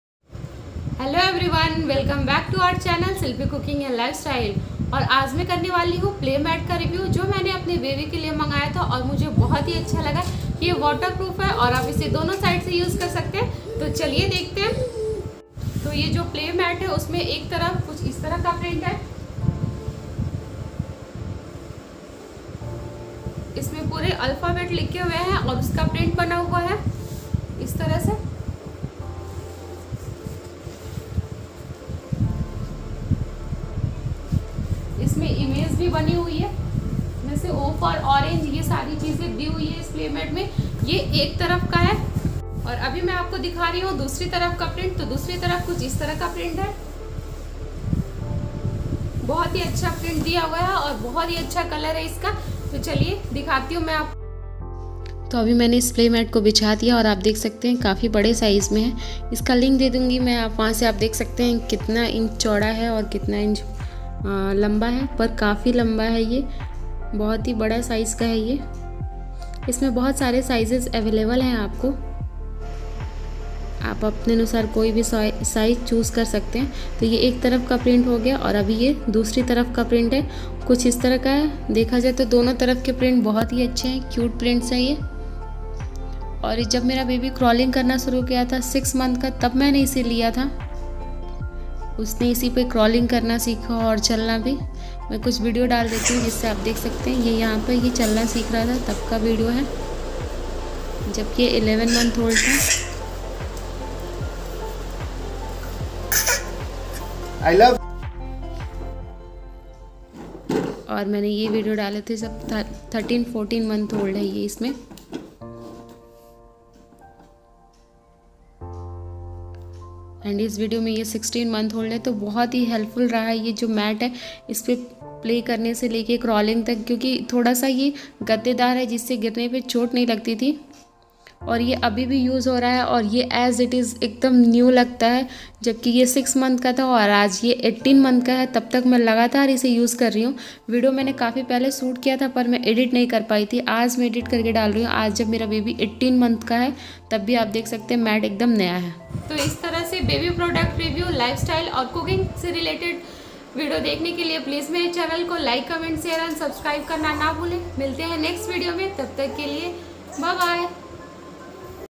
Playmate Review|Best &Cheap PlayMat Review|PlayMat under 500|Amazon
Hello everyone
it's a play mat review video, cheap and best play mat, you can buy on Amazon, Best play mat I use since my child was 6 month old and now my child is 18 month old but play mat is new , it's worth buying.

all sizes available you can choose by your use and like.